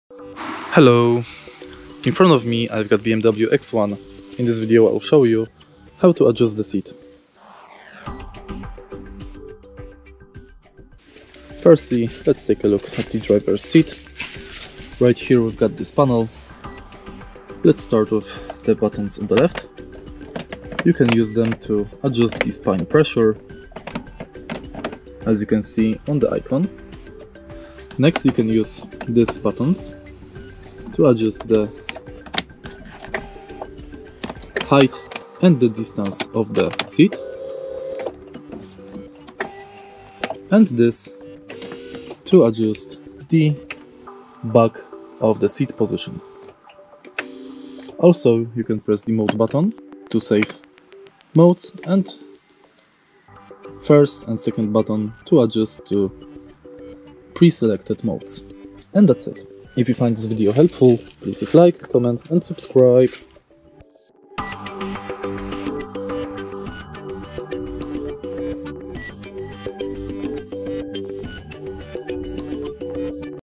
How to Change Seat Position in BMW X1 F48 ( 2015 – now ) - Adjust Seat Position
Find out more info about BMW X1 F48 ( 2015 – now ) :
Would You like to know how to adjust the seat position in BMW X1 F48 A well-adjusted seat is crucial for a safe and comfortable driving experience. Follow the video above to learn how to manage Your BMW X1 F48!

How to Adjust Seat Position? How to Change Seat Position?

Years of Production:

2015 - now

Generation:

II Gen - ( 2015 - now )

Series:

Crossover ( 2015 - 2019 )
Crossover Facelifting ( 2019 - now )
Crossover Plug-In ( 2020 - now )